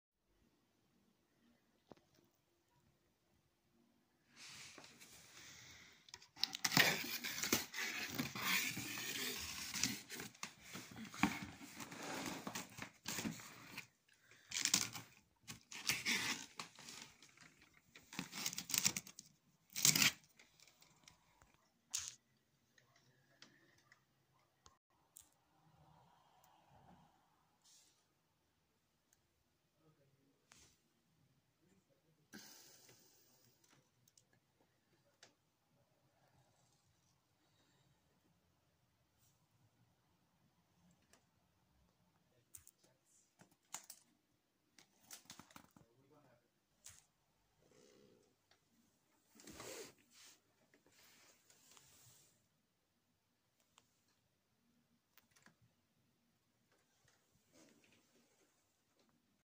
RO- comandă plante pt grădină 2021noiemrie. HU- rendeltem növényeket a kertbe. 2021 november
am comandat plante ornamentale și semințe.
au fost împachetare f.bine.
unele semințe sunt mai speciale, să vedem cu vor crește plantele la anul.
cele ornamentale sunt plantate după câteva zile de la primire.